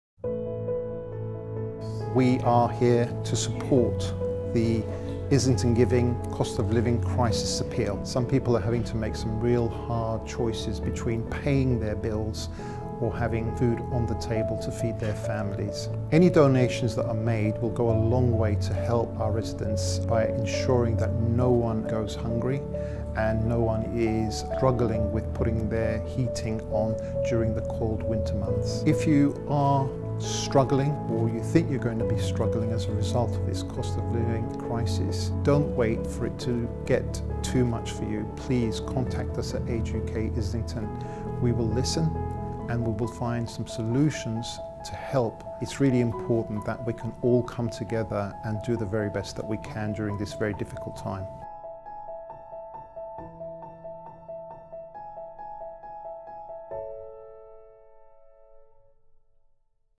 Age UK Islington - Islington Giving Cost of Living Appeal
Age UK Islington shares how Islington Giving's Cost of Living Appeal helps Islington residents in need